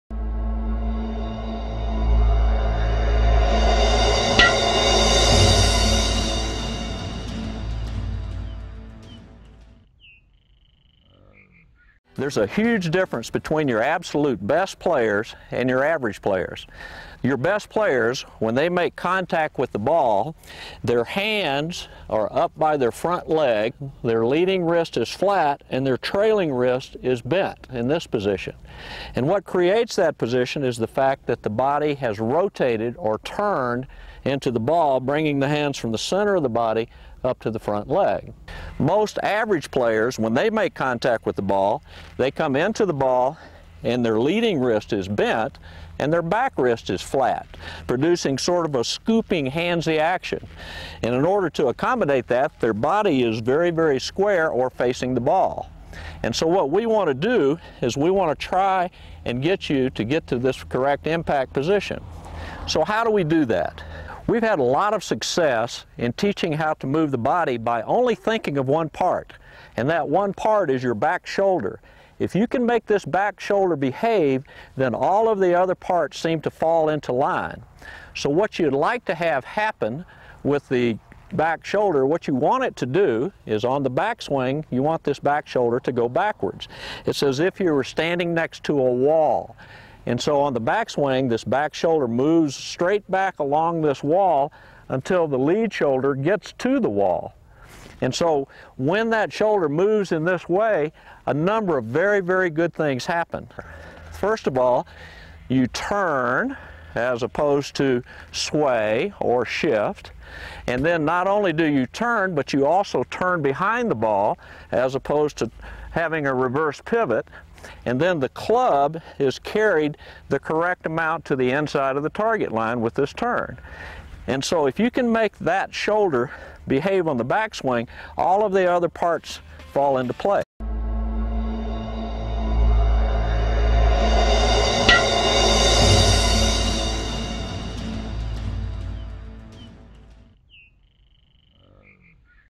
Chuck Cook: Impact Position
Golf Life Television presents a golf tip from Golf Digest Top 10 instructor and former Payne Stewert coach Chuck Cook.